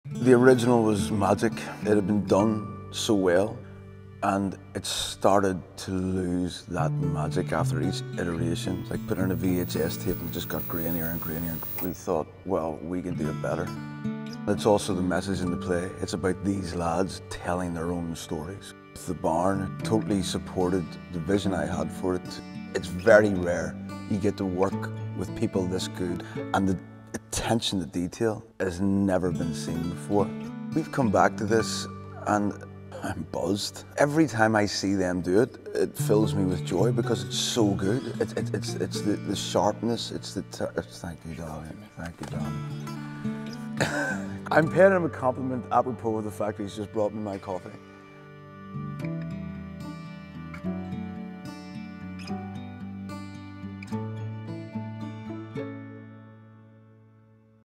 Stones In His Pockets 2024 | Matthew McElhinney On Returning To Stones
Lights! Camera! Action!! HOLLYWOOD is coming back to the Barn once again! The return of the 2021 smash hit STONES IN HIS POCKETS also went on a sellout transfer in 2022 to the Lyric Theatre in Belfast as part of a 25th anniversary celebration of the show. The new version re-opens at the Barn before going on a limited UK tour for 2024, followed by a major 2025 tour of the UK and Ireland.